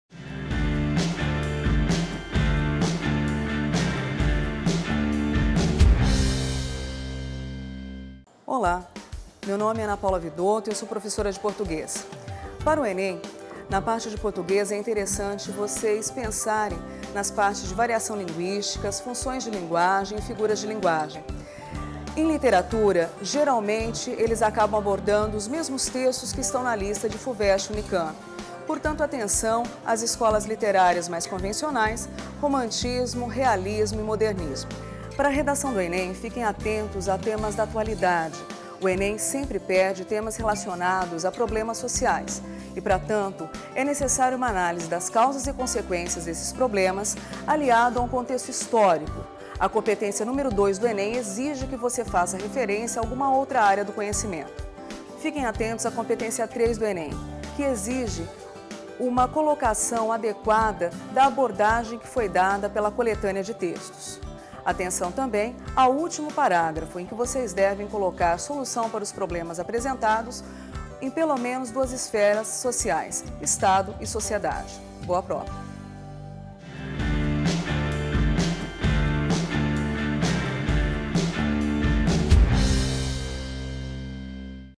TVC ENEM ANA PAULA PORTUGUES
Professora de português Ana Paula Vidotto, do colégio London, dá dicas para o Enem 2015. Outubro de 2015.